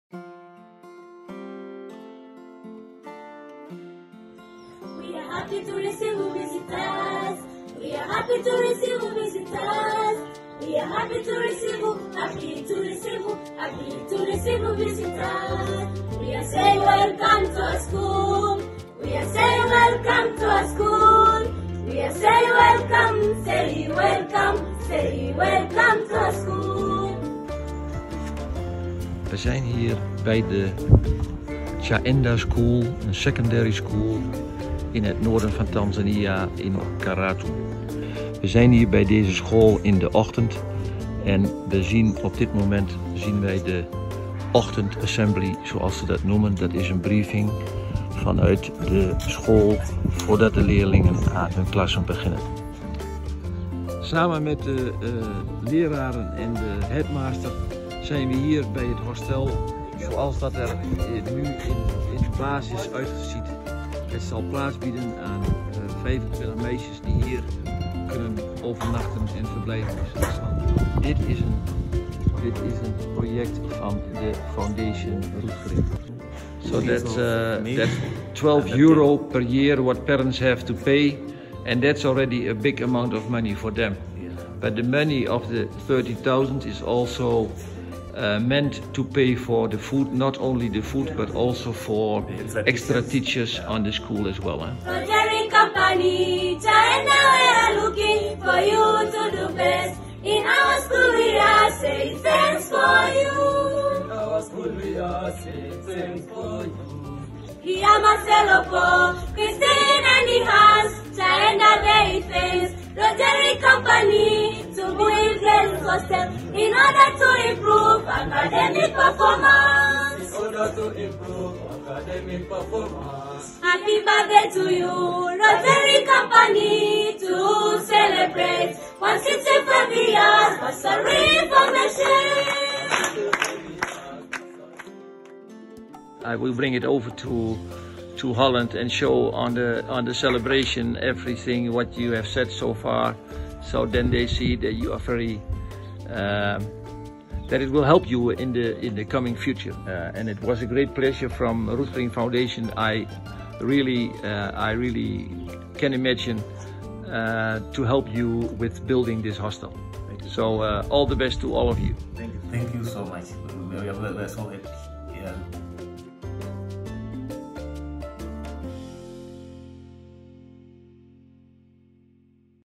Roetgerink Foundation bouw Hostel Tanzania
Niet alleen in Nederland zijn er kinderen die niet onbezorgd op kunnen groeien. In Afrika bijvoorbeeld liggen de scholen vaak zodanig ver van huis dat het voor vooral meisjes onveilig en niet haalbaar is om iedere dag naar school te gaan. Dankzij financiële steun van de Roetgerink Foundation is er in Karatu, Tanzania, bij de Chaenda Secondary School een hostel gebouwd. Het hostel biedt naar schoolgaande meisjes een veilige plek om te overnachten en van daar elke dag naar school te gaan.

Deze film laat een update van de bouw van het hostel zien. Ter gelegenheid van Roetgerink's 165 jarig jubileum reisde Dhr. H. Siers naar Tanzania om de vorderingen van de bouw te bekijken.